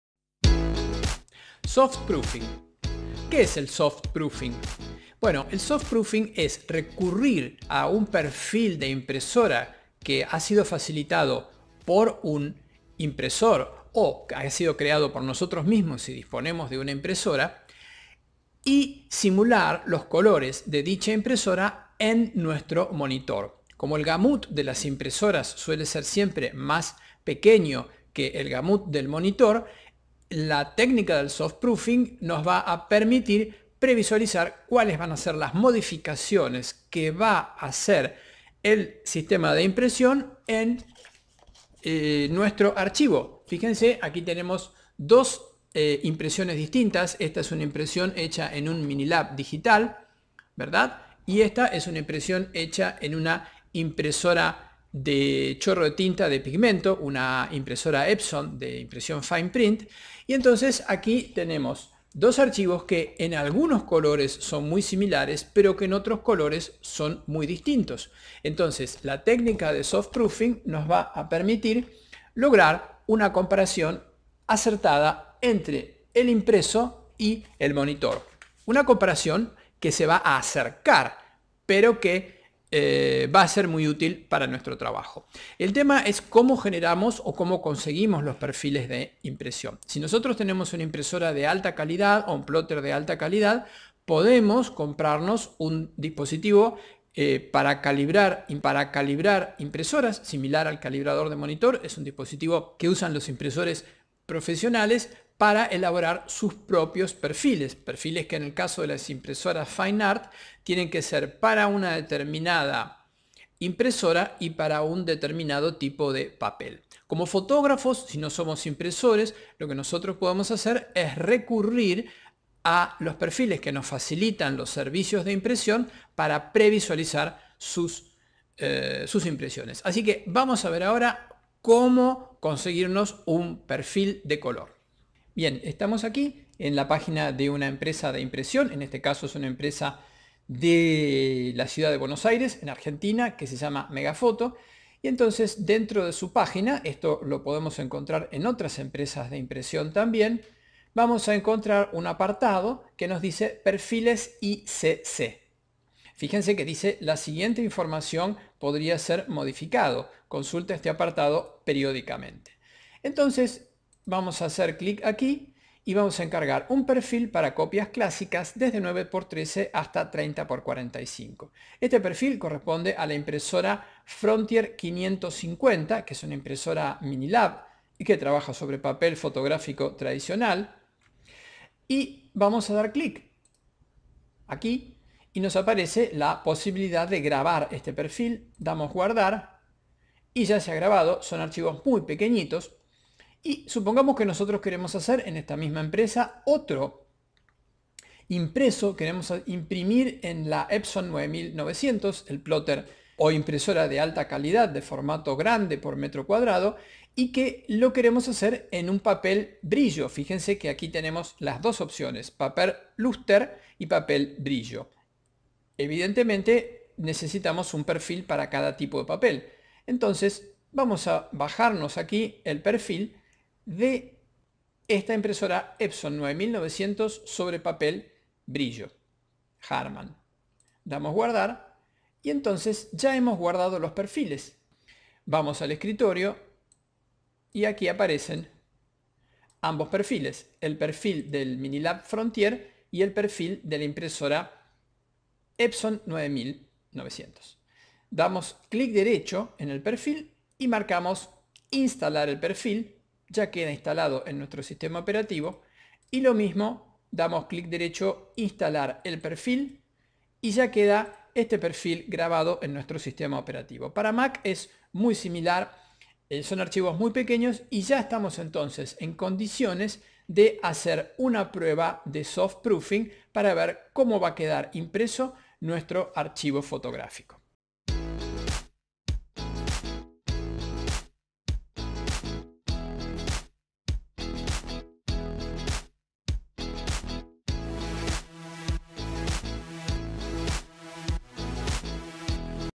Que es el Soft proofing? Cómo instalar un perfil de impresora?
El Soft Proofing consiste en simular el funcionamiento de un dispositivo de impresión en nuestro monitor.
Para esto debemos contar con el perfil de impresión que nos facilitará la empresa que imprimirá nuestra fotografía. Muchas empresas de impresión no hacen perfiles de impresión y no tienen sus equipos correctamente calibrados, mientras que otras sí. No es necesario decir que debemos elegir trabajar con estas últimas.
Una vez que hayamos obtenido el perfil, debemos cargarlo en nuestro sistema operativo.
En ciertos casos las pruebas en pantalla nos ayudarán a seleccionar, entre varias fotos, cuales son las que mejor se imprimirán. En el caso de encargos comerciales facilitarán el entendimiento con diseñadores y  clientes.
No sólo las empresas de impresión producen perfiles de impresión. Empresas productoras de papeles como Canson y Hahnemuehle se ocupan de crear perfiles de impresión para distintas impresoras Fine Print para todos sus papeles artísticos, y muchas empresas de impresión emplean con éxito estos perfiles genéricos

Te gustó este vídeo? Déjanos tu like!